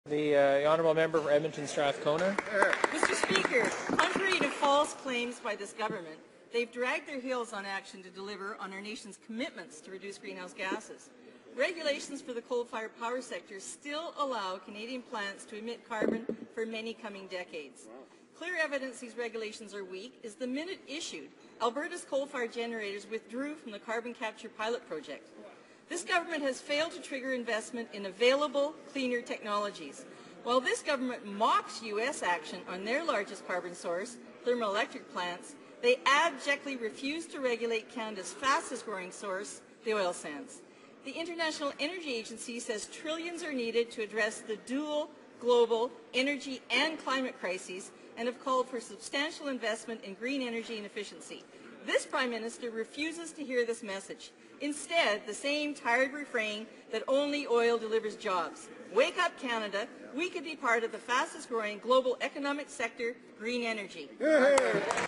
Statement - Investment in clean technologies
Mr. Speaker, contrary to false claims by the government, it has dragged its heels on action to deliver on our nation's commitments to reduce greenhouse gases.
    Regulations for the coal-fired power sector still allow Canadian plants to emit carbon for many coming decades. Clear evidence that these regulations are weak is that the minute they were issued, Alberta's coal-fired generators withdrew from the carbon capture pilot project. The government has failed to trigger investment in available cleaner technologies.
    While the government mocks U.S. action on its largest carbon source, thermal electric plants, it abjectly refuses to regulate Canada's fastest-growing source, the oil sands.
    The International Energy Agency says that trillions are needed to address the dual global energy and climate crises, and has called for substantial investment in green energy and efficiency. The Prime Minister refuses to hear this message. Instead, it is the same tired refrain that only oil delivers jobs.
    Wake up, Canada. We could be part of the fastest-growing global economic sector: green energy.